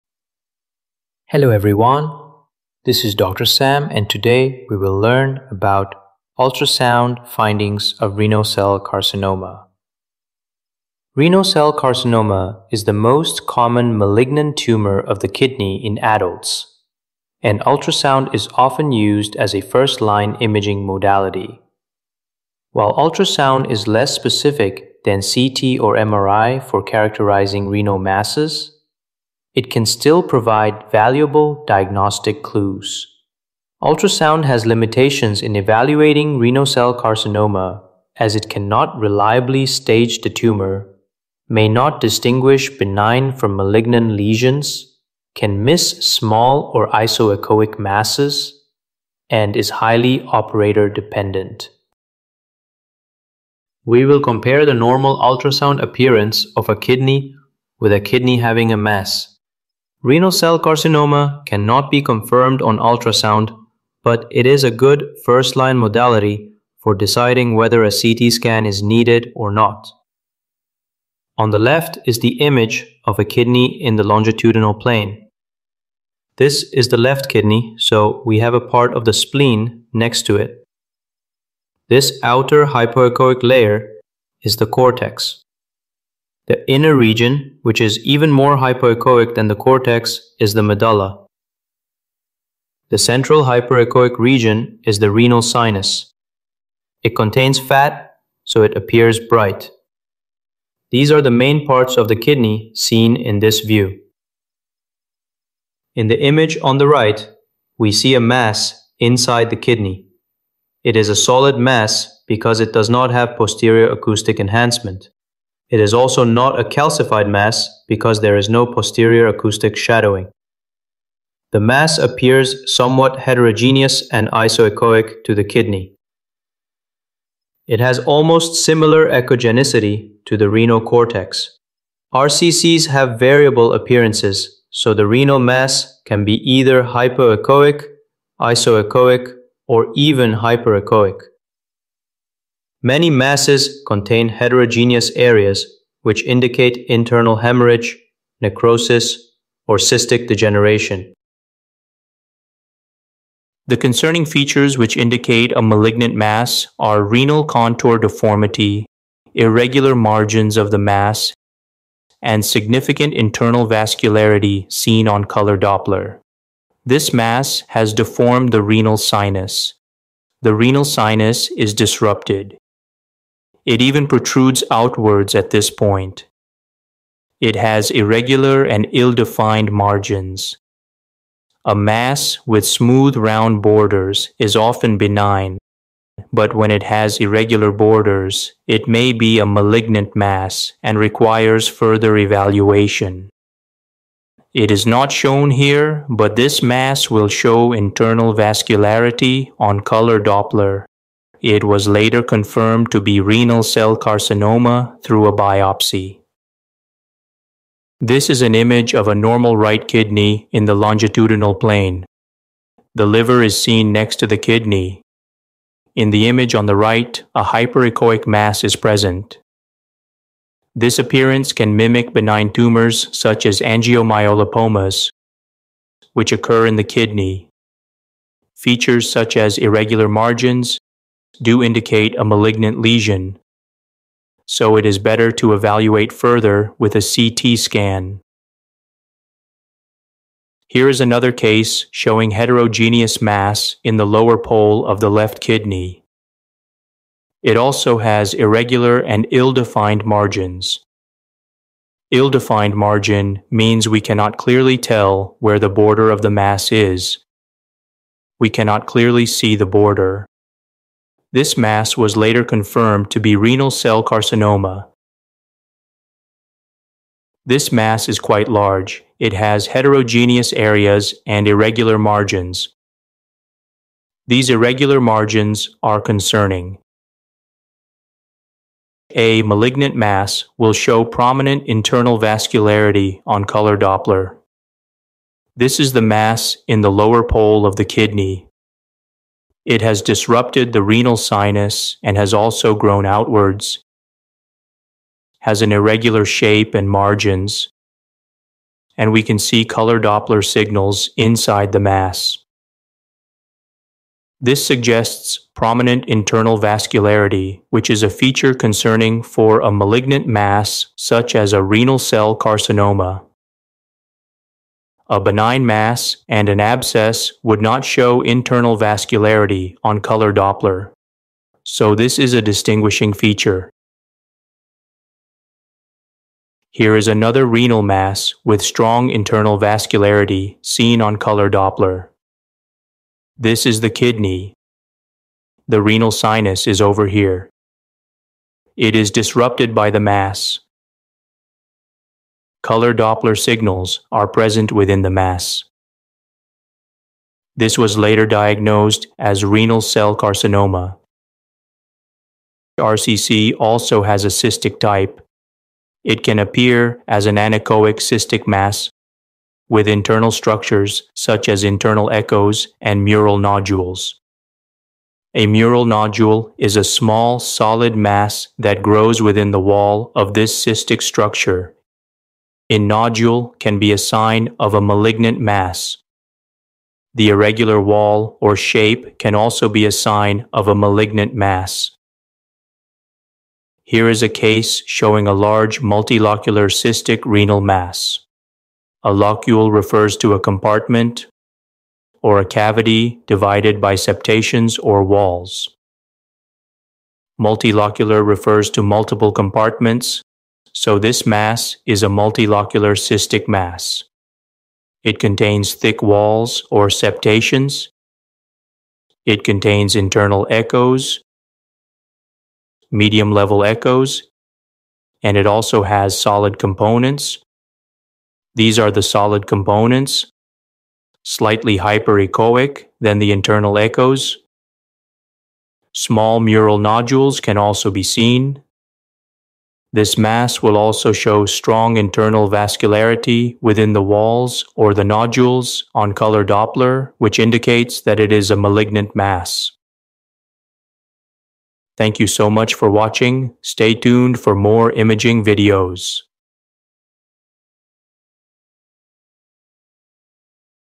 Kidney Ultrasound Normal Vs Renal Cell Carcinoma Image Appearances | Malignant Renal Cancers RCC USG
RCC Ultrasound Limitations:
Cannot reliably stage the tumor.
May not differentiate benign from malignant in certain cases.
May miss small or isoechoic lesions.
Operator-dependent

Renal Cell Carcinoma:
Solid Renal Mass: Hypoechoic, Isoechoic, or Hyperechoic
Heterogeneous Echotexture of the mass: Due to internal hemorrhage, necrosis, or cystic degeneration
Renal Contour Deformity: Tumor may distort the normal renal shape.
Irregular Margins: May have poorly defined or lobulated borders.
Increased Internal Vascularity: Seen on color Doppler.
Chaotic or Disorganized Blood Flow: Irregular, non-uniform vascular architecture.

Cystic RCC:
Cystic mass
Mural Nodule
Irregular wall/shape

Multilocular Cystic RCC:
Thick septations.
Solid components.
Mural nodules.
Irregular walls.
Internal vascularity within septa or nodules.